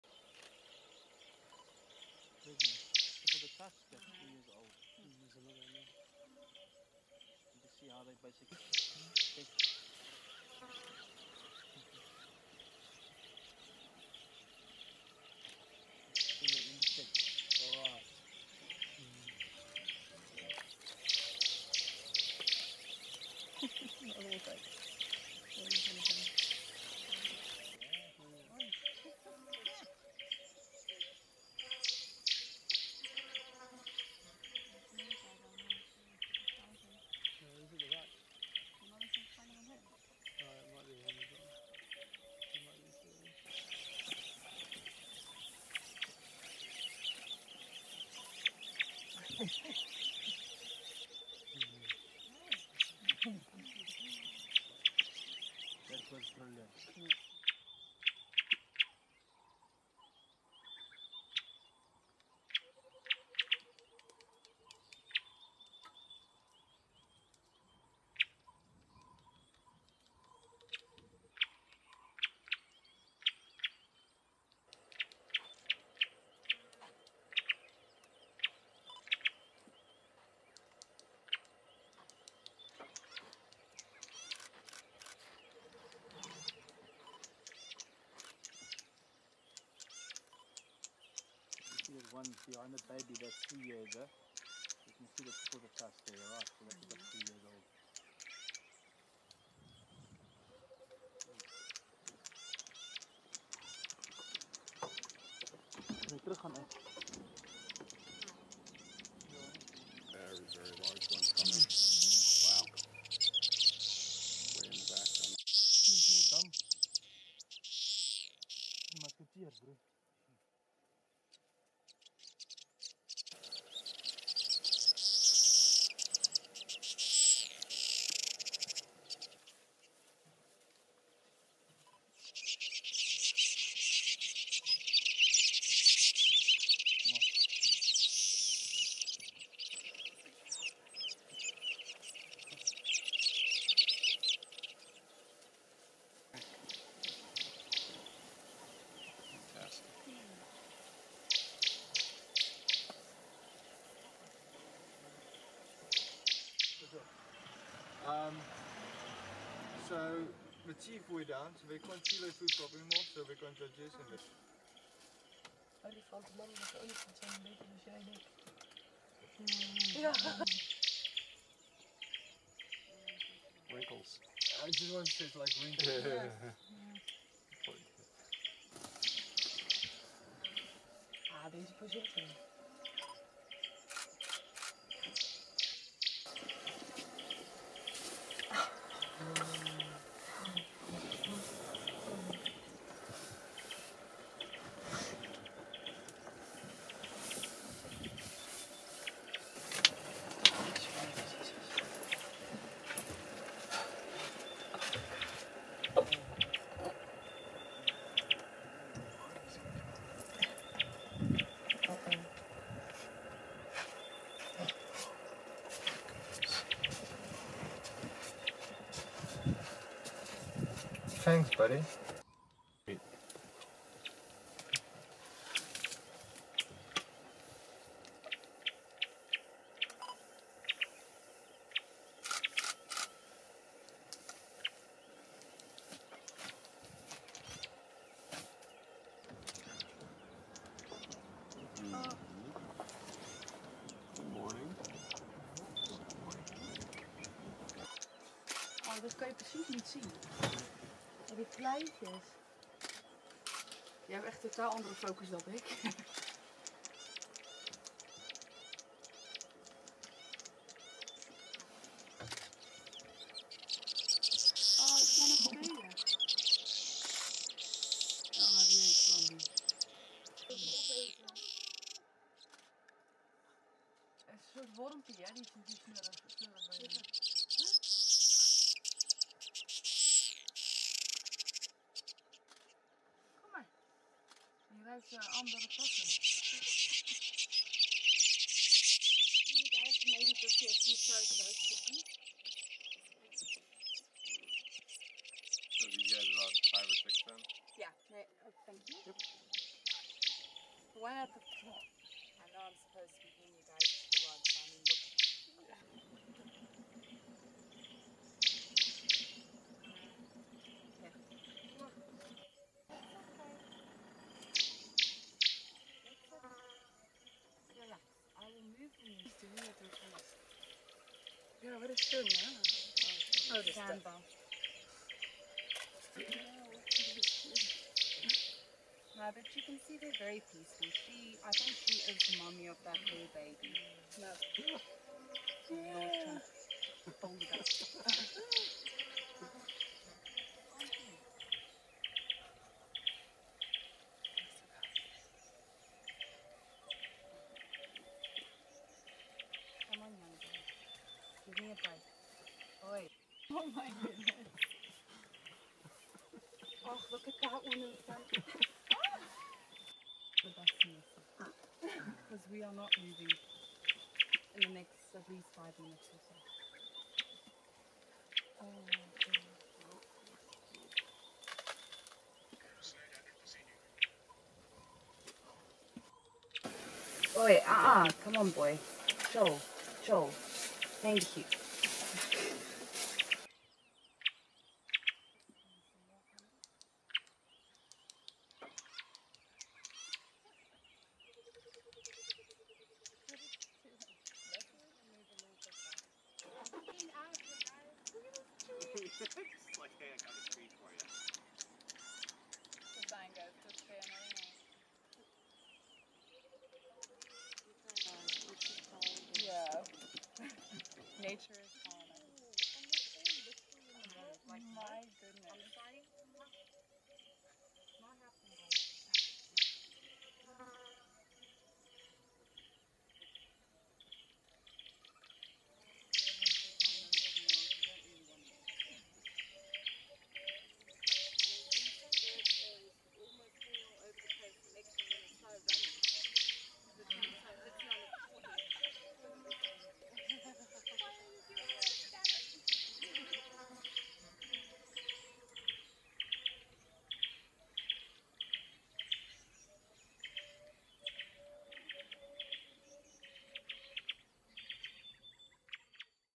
great elephants in sabi sands and kruger national park - close encounters
watching some big herds of elephants very close in sabi sands and kruger national park, elefantenherden, ganz nah, very close